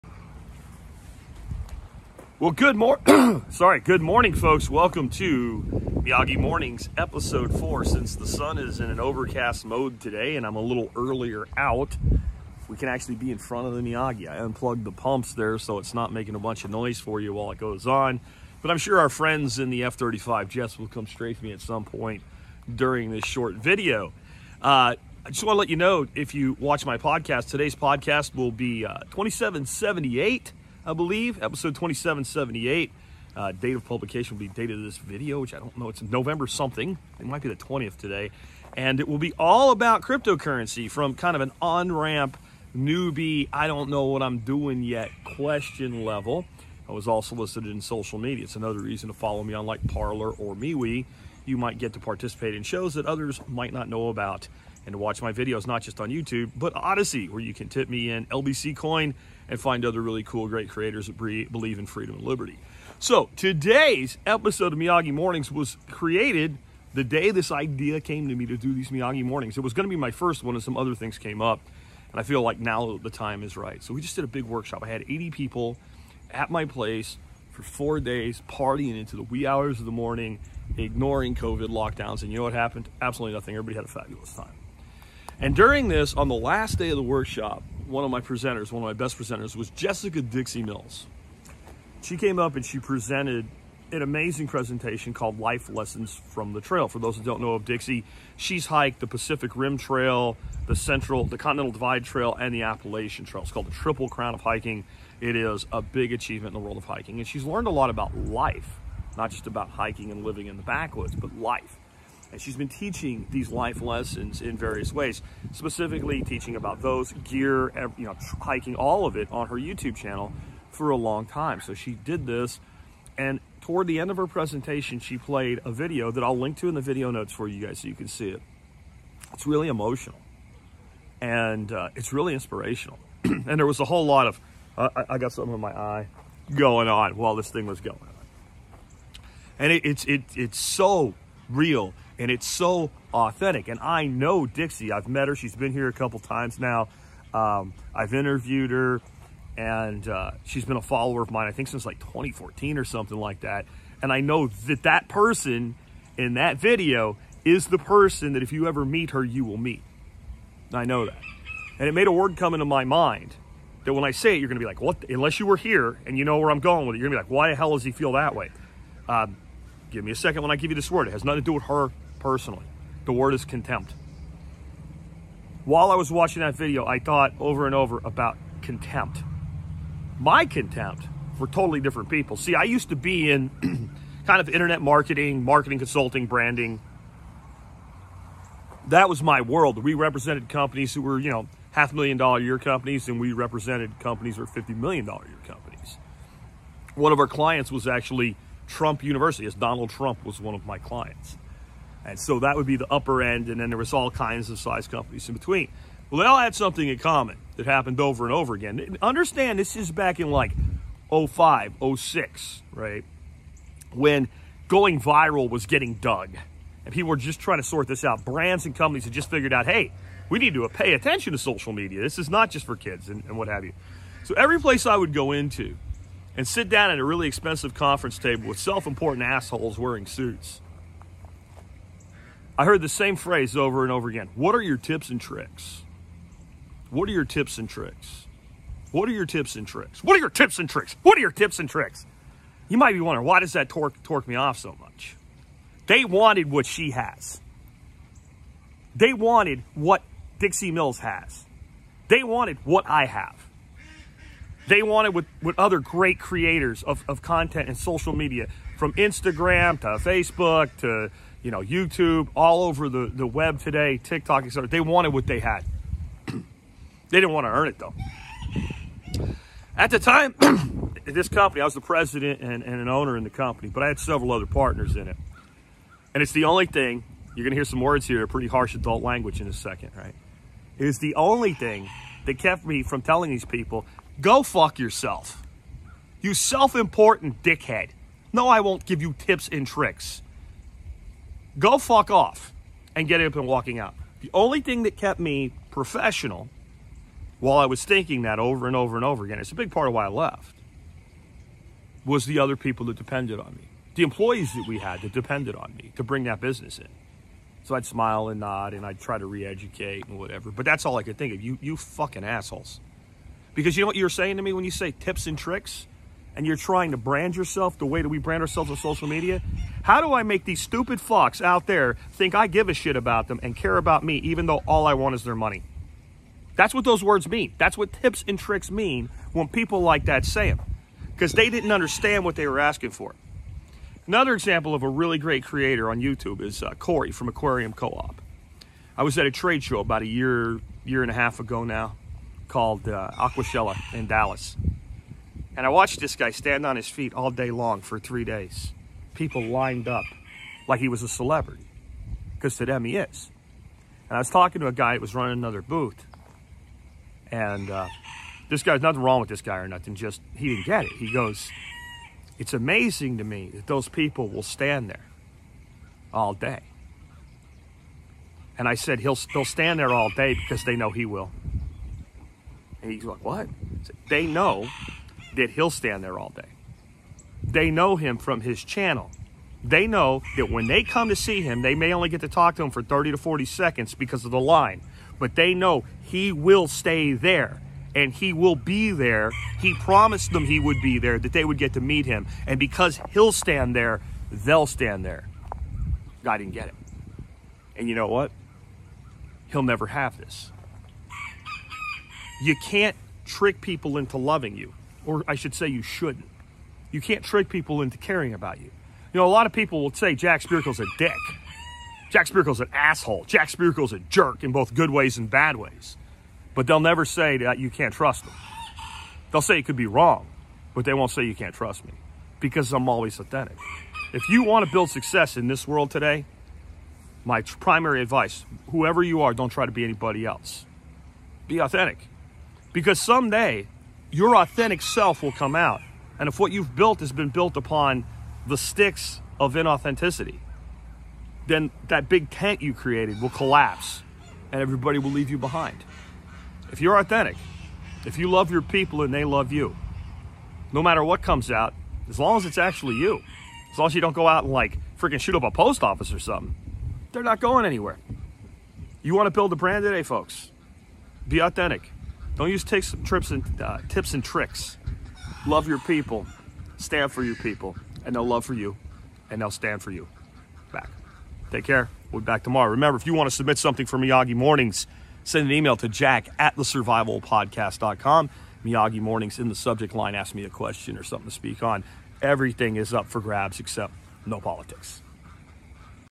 Screw "Tips and Tricks" Be Authentic - Miyagi Mornings #4 - Adult Language Warning
If you like this video and this type of thing you will love my podcast the podcast mentioned in this episode will be Episode 2777 and will go live about 3PM Central on 11-19-20 and will be about getting started with cryptocurrency.

This is the video referenced in this video that Dixie played at my workshop and here is her channel.

Today I talk about one of the main reasons I quit doing consulting work for corporate America.  Adult language warning on this one.